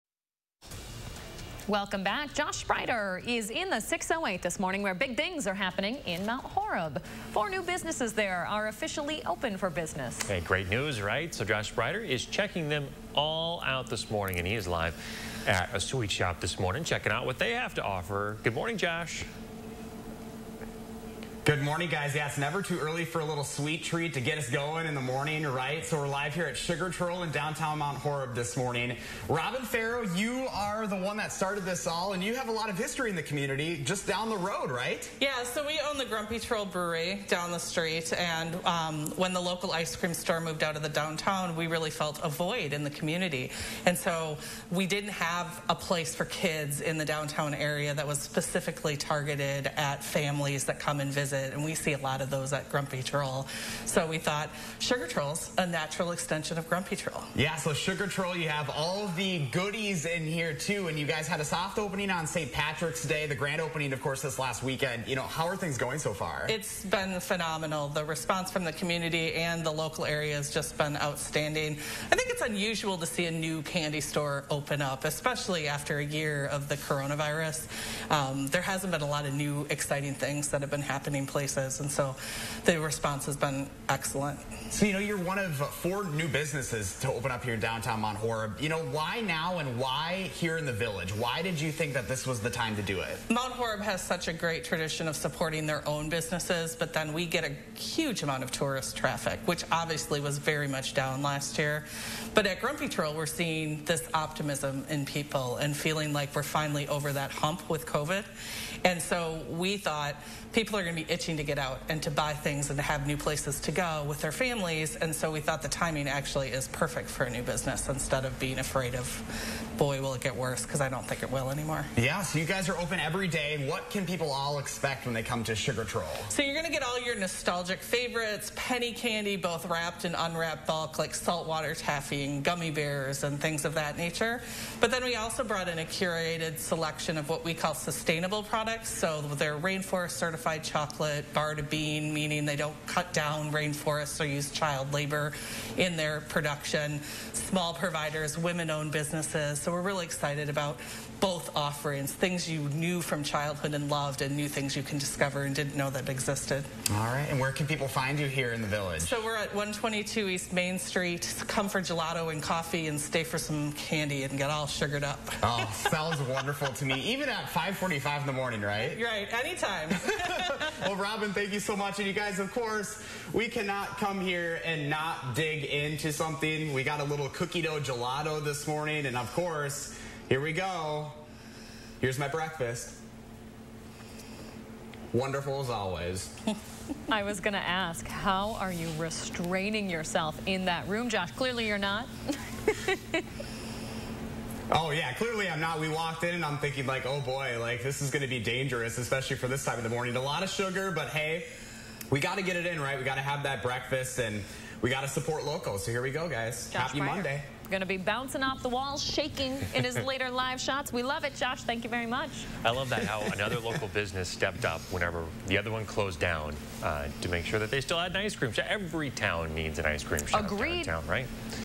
In the 608: Business booming for downtown Mount Horeb
Sugar Troll, which has the same owner as Grumpy Troll Brewpub, is Mount Horeb’s first candy shop. Guests can find gelato, fudge, macarons, taffy, vintage sweets and craft treats. Sugar Troll is located at 122 E. Main St.